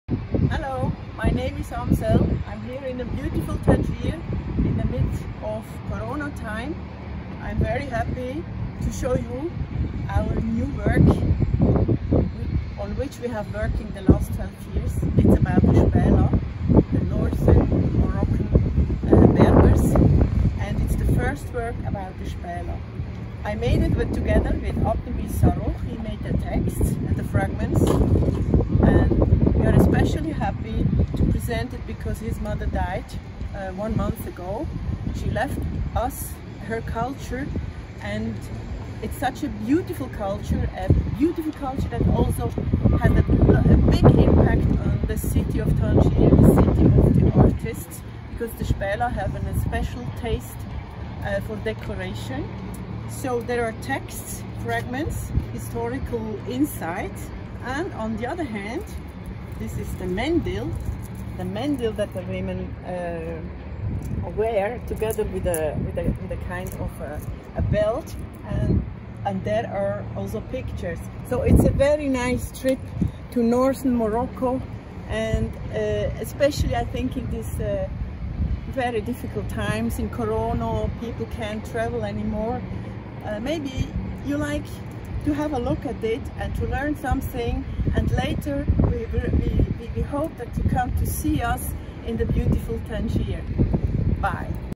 JBALA (book, livre), PHOTOS & TEXTS by Amsél & Abdenbi Sarroukh, TILL SCHAAP EDITION, BERN, 2020
PHOTOBOOK / LIVRE DE PHOTOGRAPHIE (E / E) & ESSAYS, SHORT STORIES (E)
During more than twelve years we had the honour and the chance to accompany a village in the Jbala, the region of the North-African Berbers in Morocco. Photographs and texts intend to preserve a beautiful culture, than may be in danger to disappear in the future.

Pendant douze ans nous avons eu la chance et l'honneur d'accompagner un village dans la Jbala, chez les berberes au nord du Maroc. Nos photographes et textes ont le but de préserver un style de vie et de culture, qui est en danger de disparaitre dans le future.